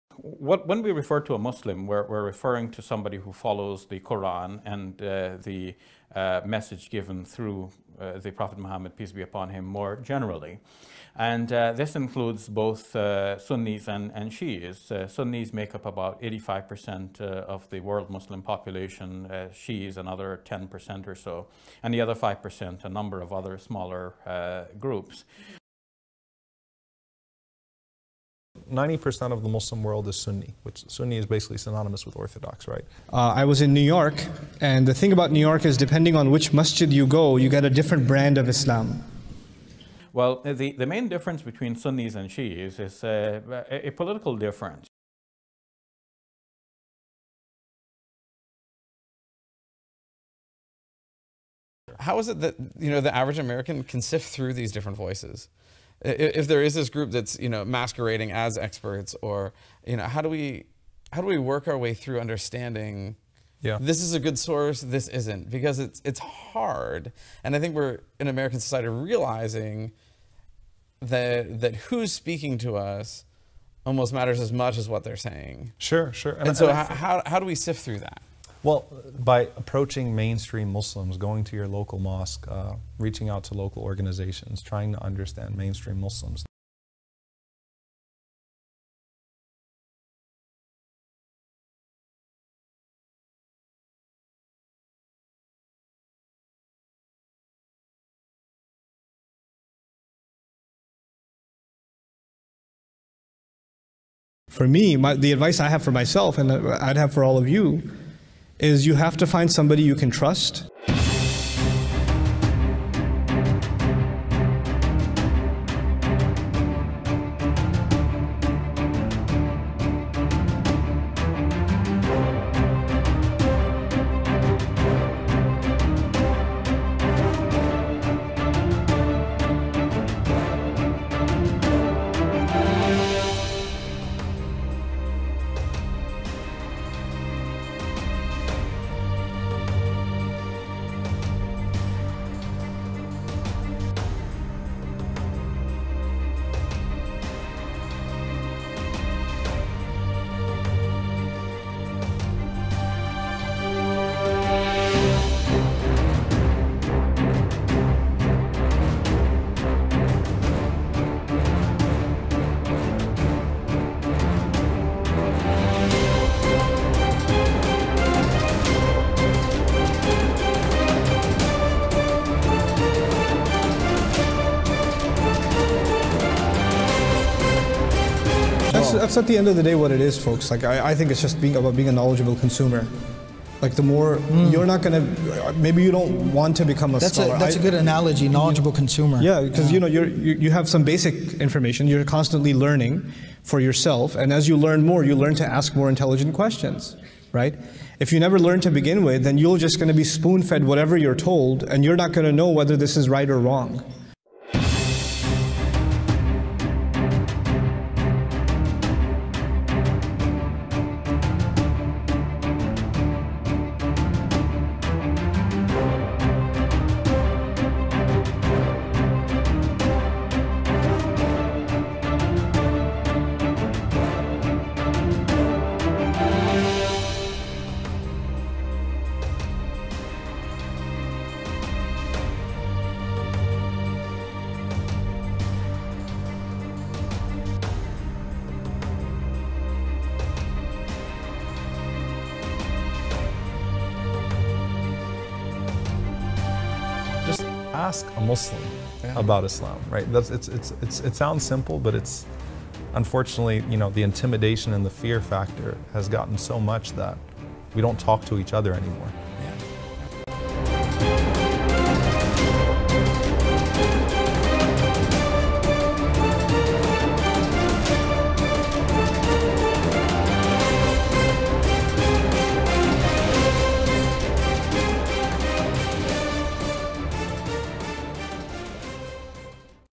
Allah (Quran) Exposes ALL Sunni and Shia "scholars". No Sectarian in Islam
Allah forbade all sectarian names in the Quran itself but so called "scholars" like Omar Suleiman, Nouman Ali Khan and Shabir Ally will never tell you that.